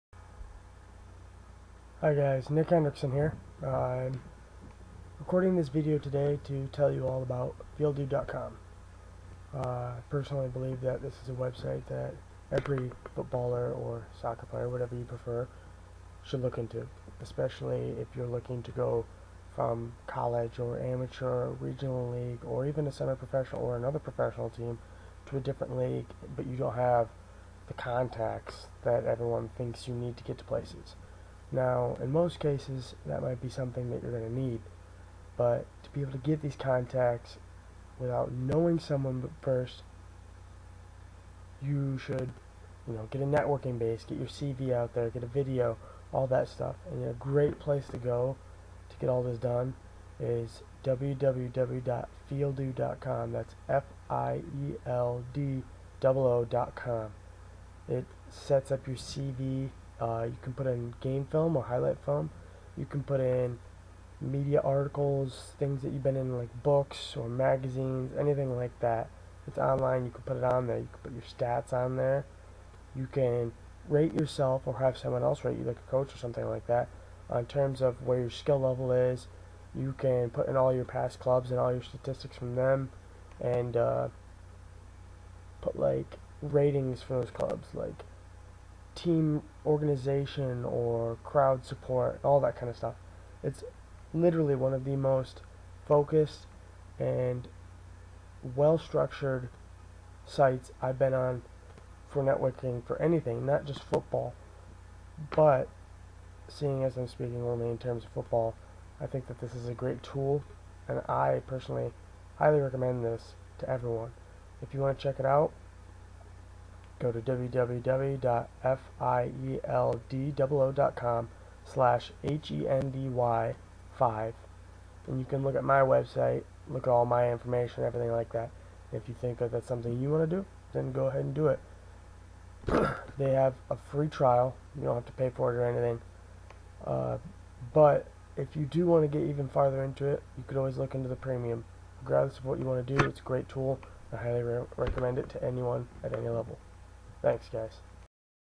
Fieldoo review.
and what I think of it in terms of its' value for networking in the field of footballers.